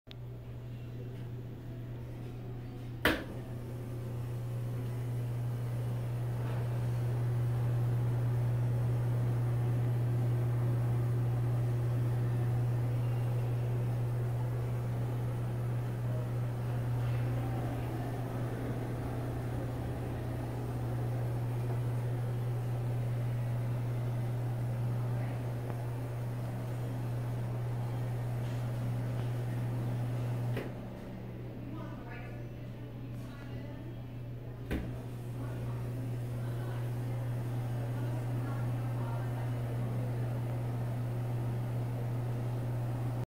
Quiet Broan exhaust fan, but has a buzz to it.
Quiet Broan exhaust dan with buzzing noise at a brewery. This fan works really good despite that it is buzzing.